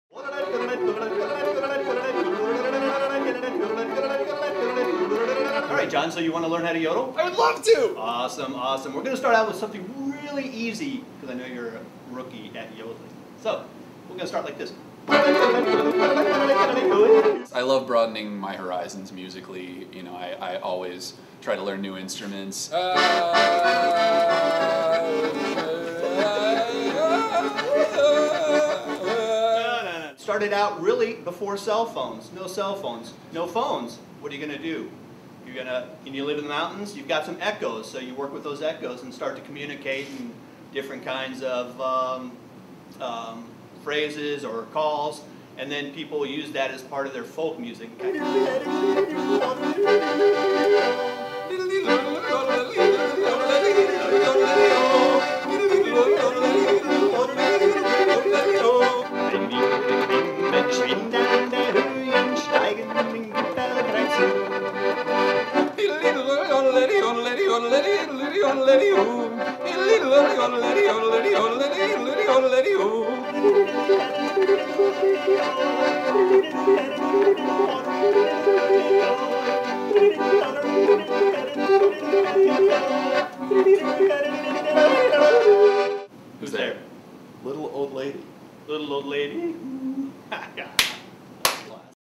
Yodeling 101
Expert yodeler Norman Gwaltney teaches rock musician John Orr the finer points of Alpine tongue acrobatics in advance of the Original and Fabulous GermanFest, scheduled Saturday, Oct. 10, at the Athenaeum, 401 E. Michigan St., Indianapolis.